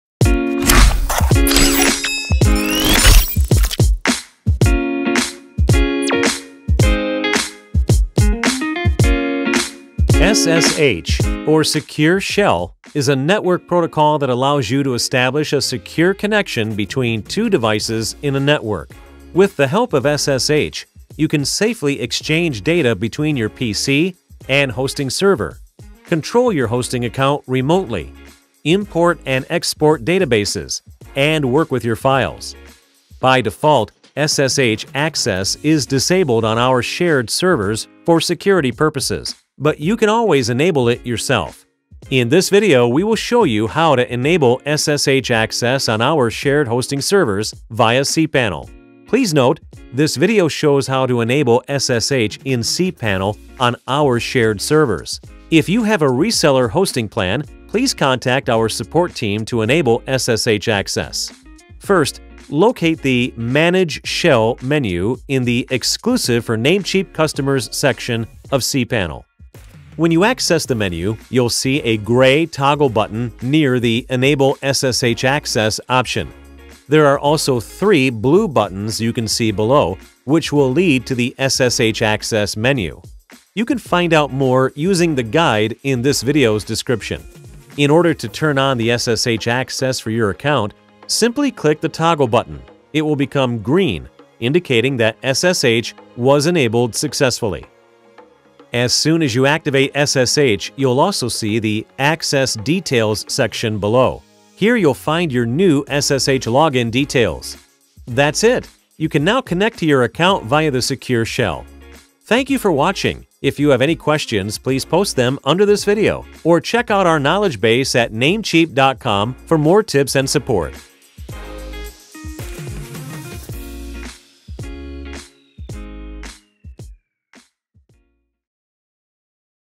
How to enable SSH in cPanel on Shared Hosting
This video shows how to enable SSH in cPanel on our Shared servers. If you have a Reseller hosting plan, please contact our Support Team to enable SSH access.